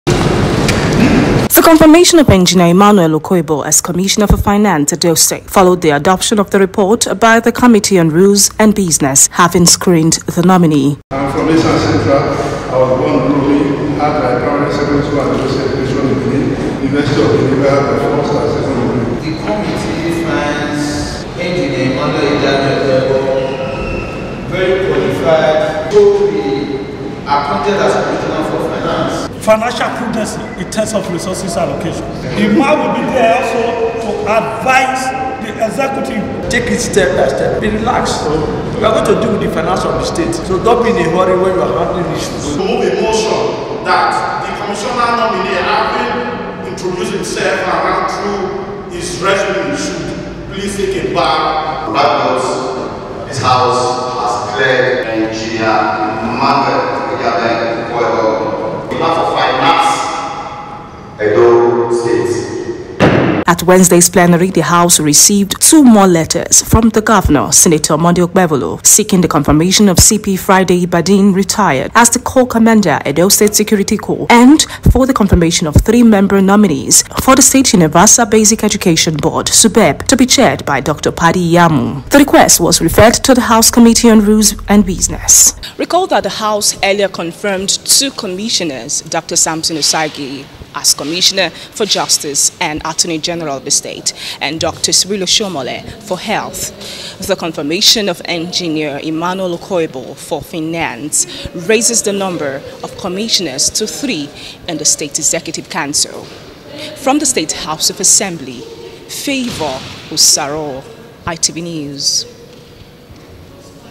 EDHA Clears Engr. Emmanuel Okoeboh As Finance Commissioner, Receives More Nominees
Edo state house of Assembly on Wednesday confirmed Engr Emmanuel Okoebor as Finance commissioner following his nomination by Governor Monday Okpebholo.

Assembly correspondent Favour Osaro reports that the house also received two additional letters of confirmation of nominees.